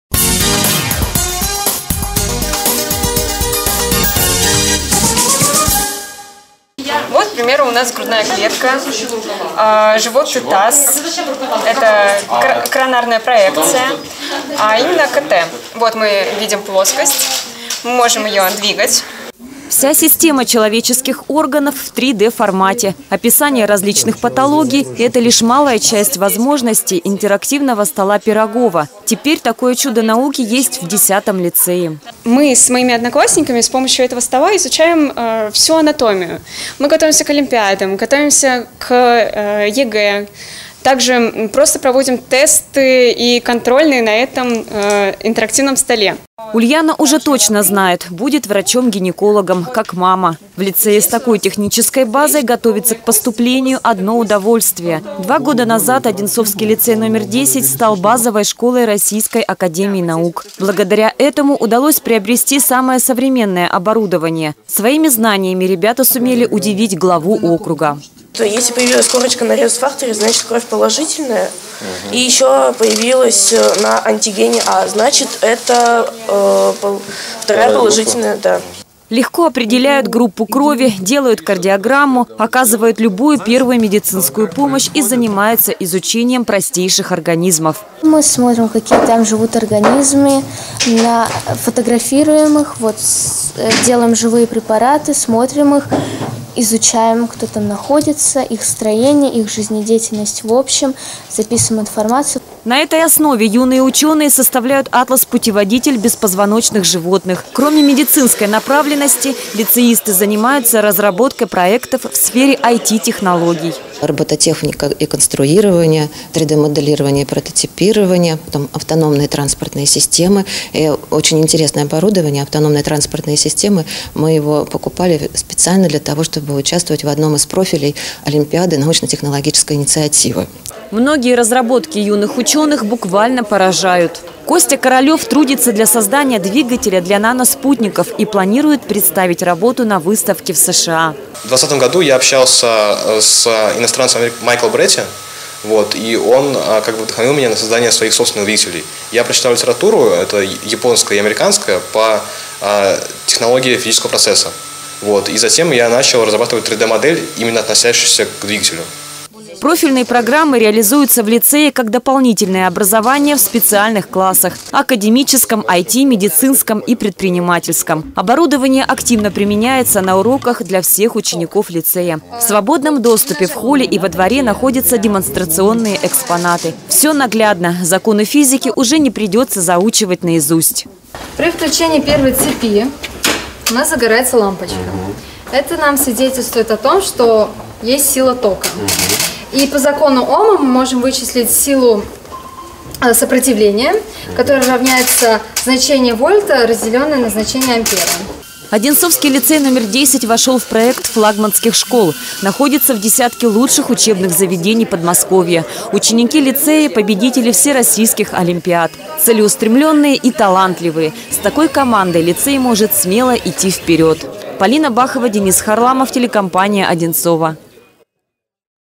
Визит главы в лицей №10
Глава муниципалитета Андрей Иванов посетил одинцовский лицей №10-один из лучших в регионе.